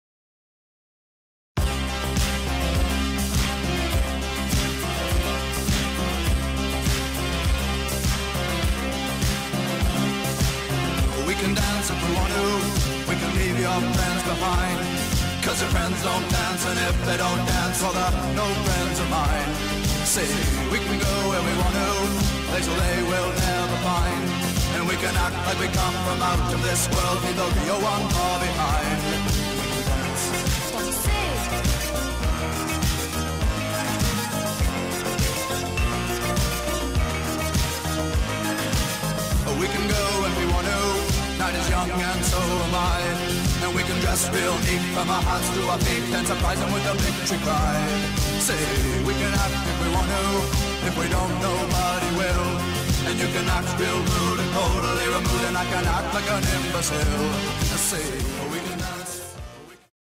2013 Cam Dance
Cam's signature dance move: The Martian Hula Hoop!